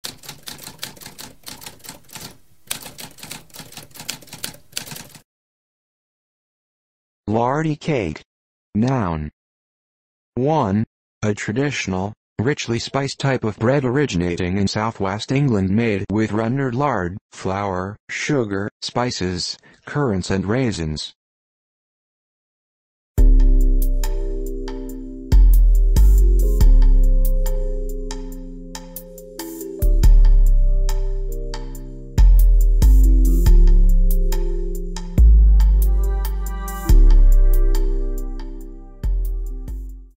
What does lardy cake mean?
A spoken definition of lardy cake.

Intro Sound:
Typewriter - Tamskp